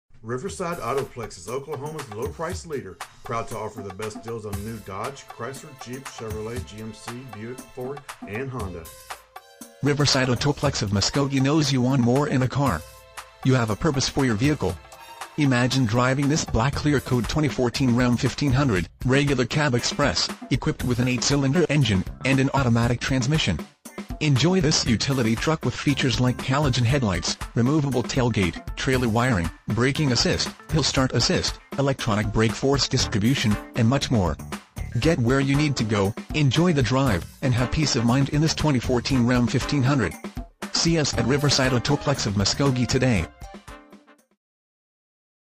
2014 RAM 1500 Riverside Autoplex of Muskogee Muskogee, OK 74401
RAM Riverside Autoplex of Muskogee knows you want more in a car. You have a purpose for your vehicle. You will love this Black Clear Coat Ram 1500 Regular Cab, equipped with an 8 Cylinder engine and an automatic transmission. Enjoy this utility truck with features like Halogen Headlights, Traction Control With Trailer Stability Assist, Removable Tailgate, Anti-Theft Alarm System With Engine Immobilizer, Trailer Wiring, Emergency Braking Assist, Hill Start Assist, Electronic Brakeforce Distribution, Front Fog Lights, Rolling Code Security, and much more. Get where you need to go, enjoy the drive, and have peace of mind in this Ram 1500. See us at Riverside Autoplex of Muskogee today!
MILES:     15
VIN:3C6JR6AT3EG240305

1711 West Shawnee
Muskogee, Oklahoma 74401